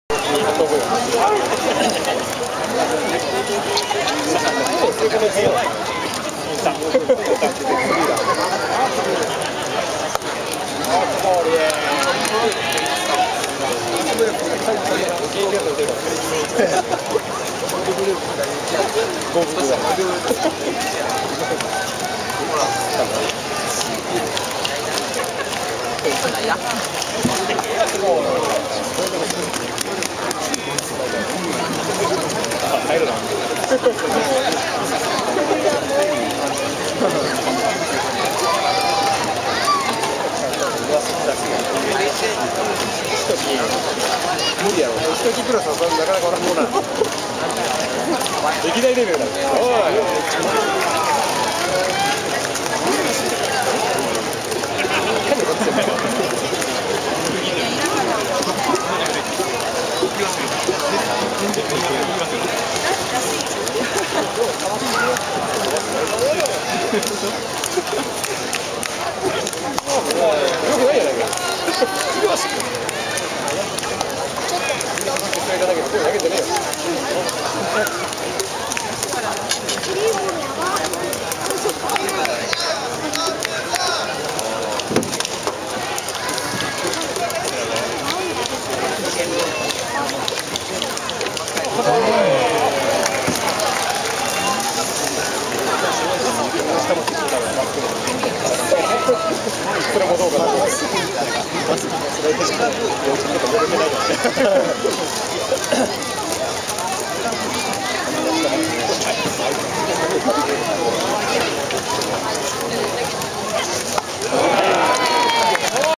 【2023 ウ・リーグ】広島カープ 田村 俊介のバッティングフォーム_2023072903
2023年ウエスタンリーグ、阪神タイガース VS 広島東洋カープ@レクザム ボールパーク丸亀（丸亀市民球場）での広島カープ 田村 俊介のバッティングフォームです。